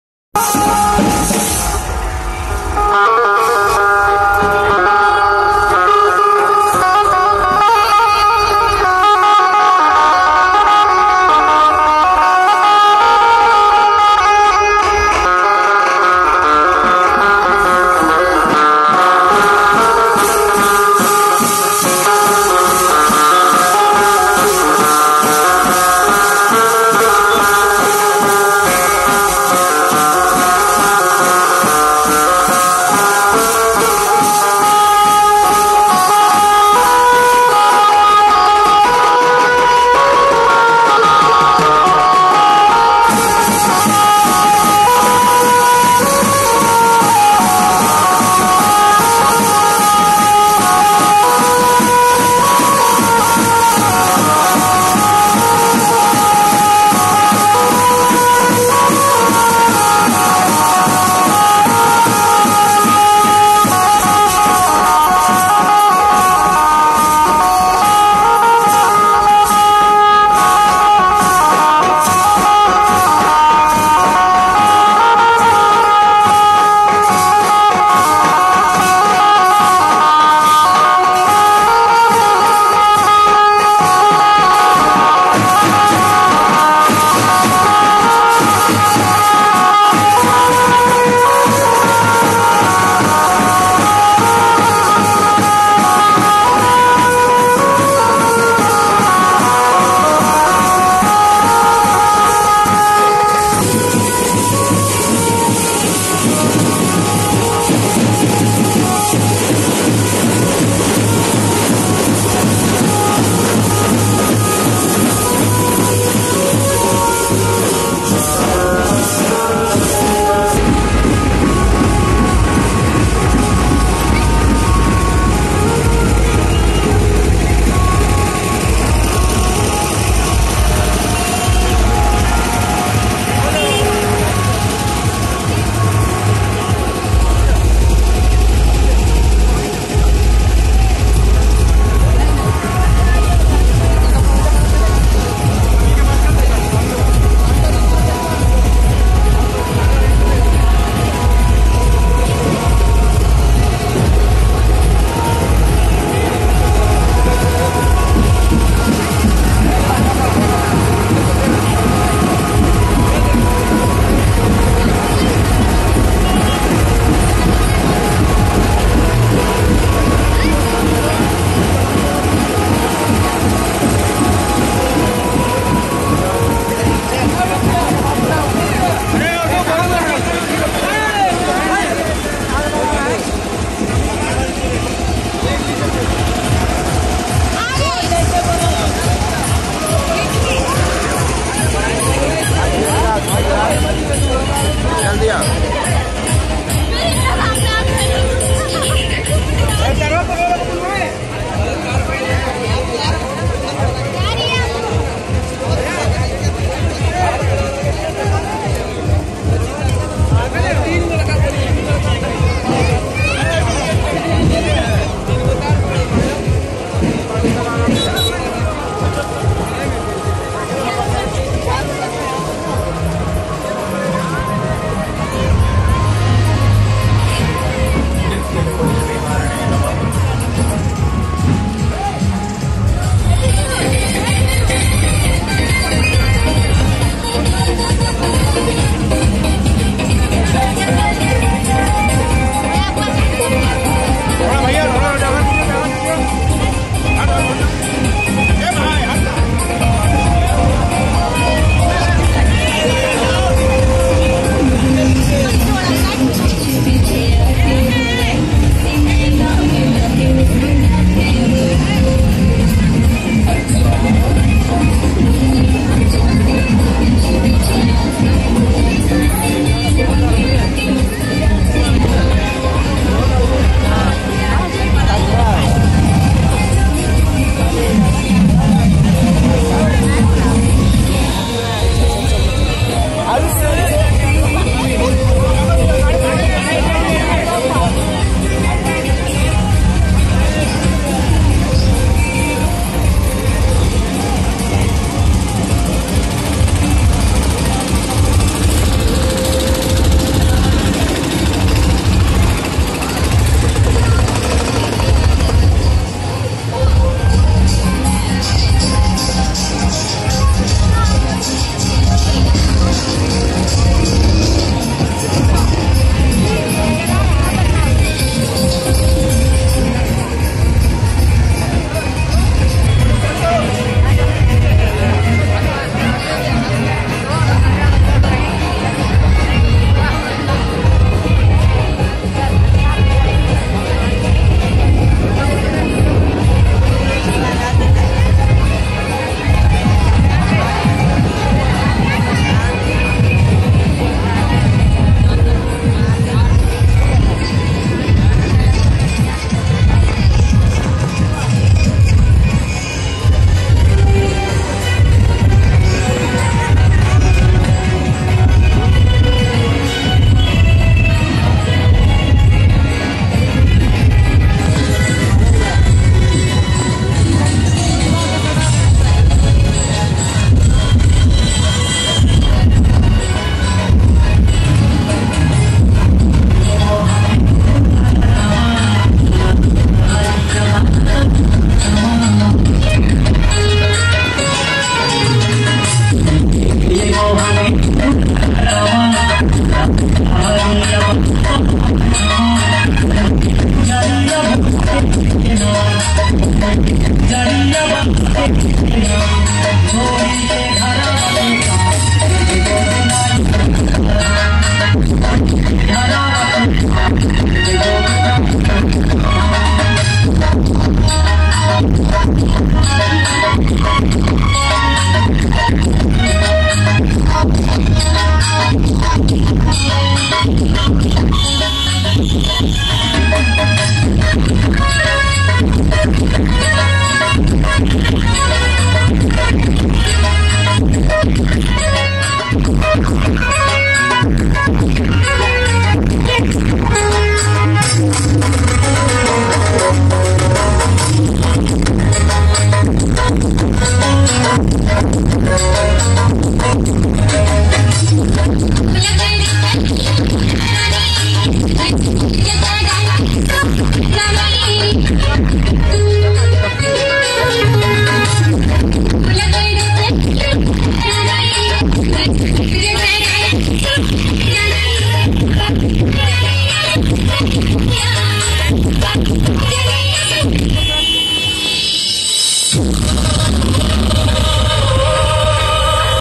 Dj Sarzen roadshow 🎉🎇 holi milan samaroh 2023 jharia
iss saal bhot harsho ullash ke sath holi milan samaroh manaya gaya ☺️ jharia jharianews dhanbad_youtuber dhanbad djsarzen djsashidhanbad djsashijharkhand djsarzenprodution youtuber youtube @bcdzzz @PKVictor